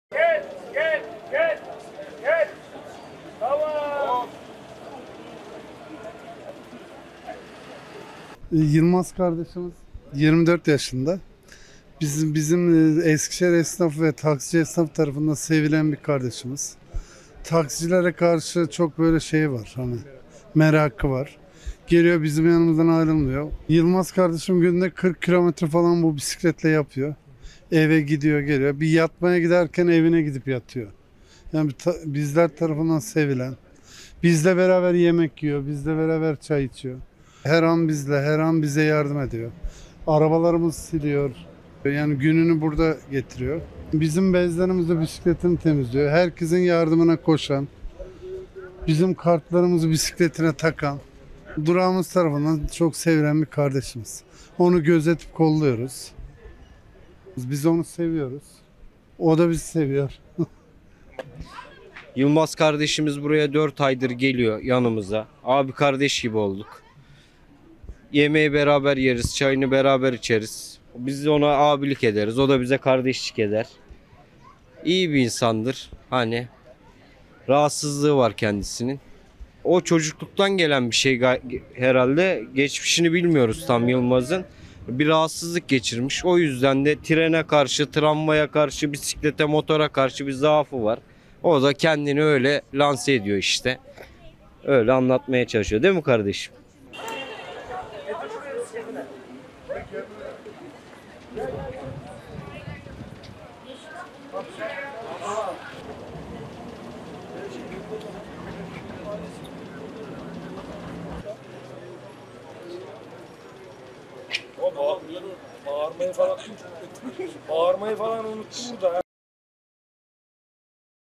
Bisikleti İle Taksici Gibi Mesai Yapıyor
Eskişehir’in Çamlıca Mahallesi’nde yaşayan Yılmaz Yaşar, her gün bisikleti ile 40 kilometre yol kat ediyor. Küçük yaşlarda geçirdiği ağır hastalıktan sonra konuşma yetisini kaybettiği öğrenilen Yaşar, şehir merkezinde bulunan Santral sokaktaki taksici esnafı ile olan ilişkisi görenlerin içini ısıtıyor.